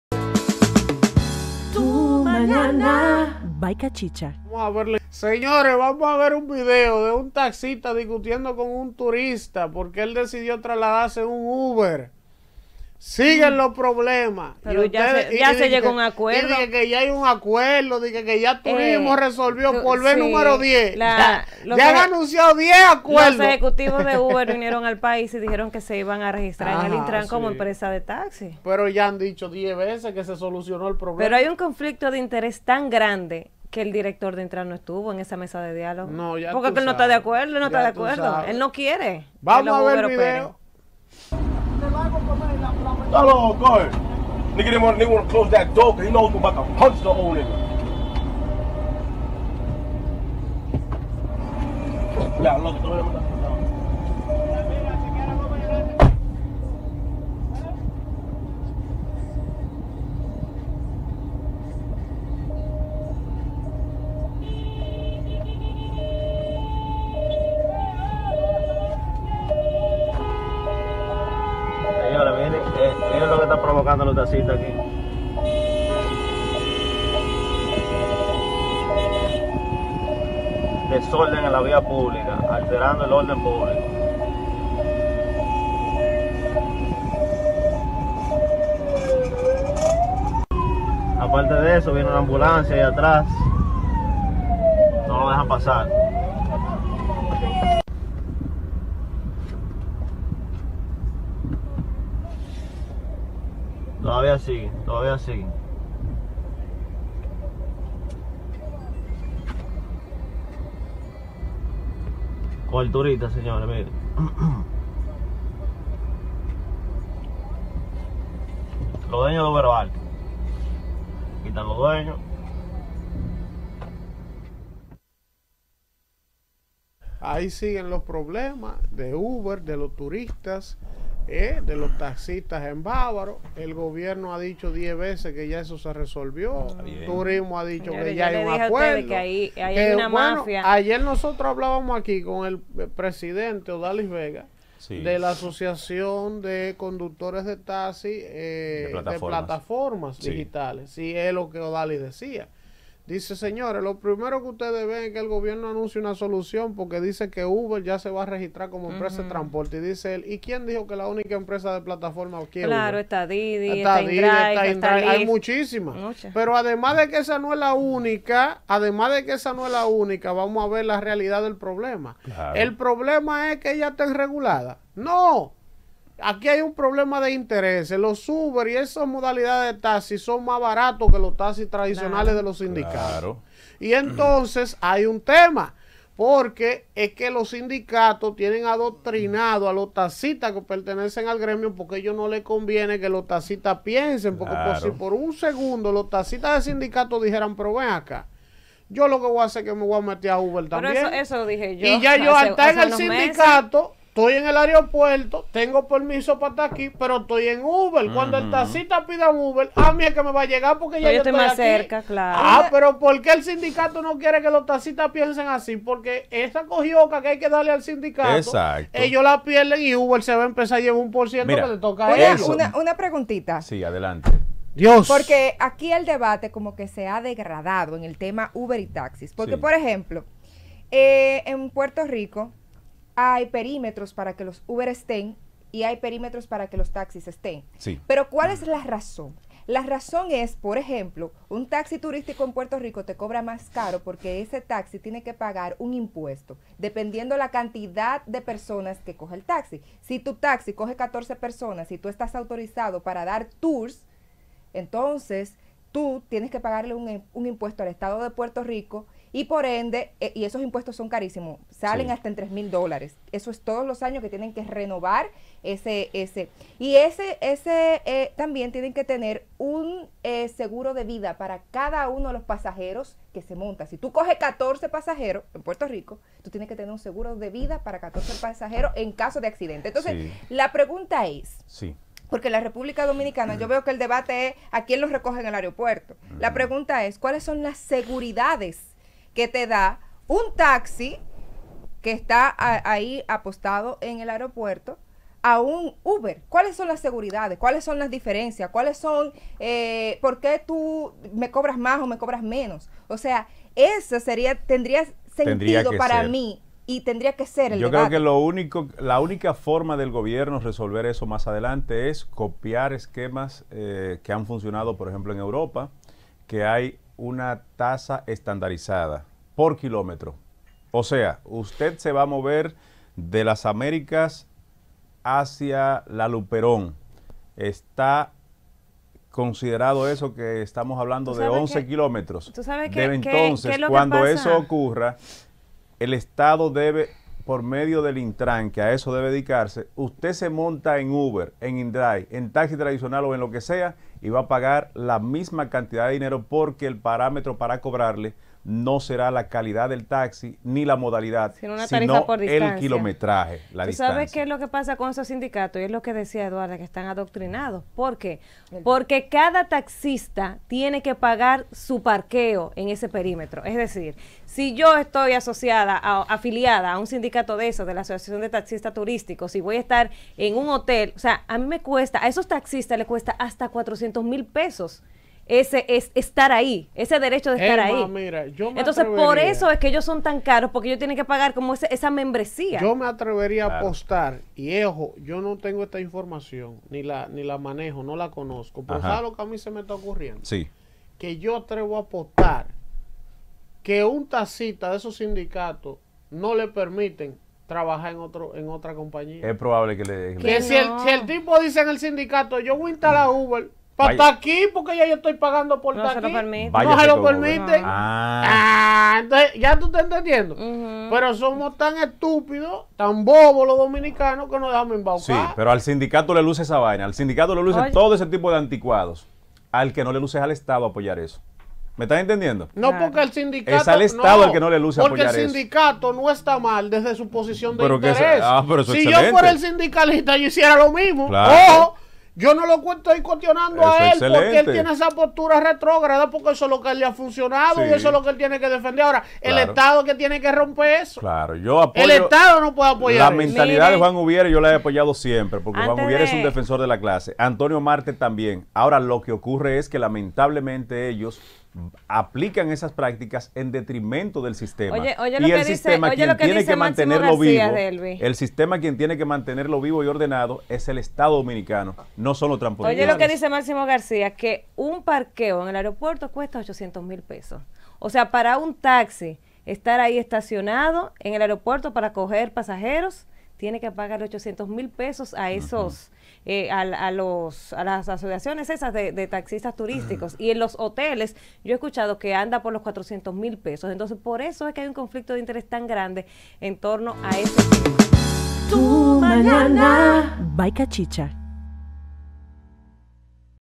Taxistas siguen de imprudentes, a pesar del acuerdo con UBER | Tu Mañana By Cachicha
Luego de que el gobierno haya llegado a un acuerdo con UBER para evitar los conflictos con los taxistas, los mismos siguen de imprudentes, haciéndole daño al turismo dominicano.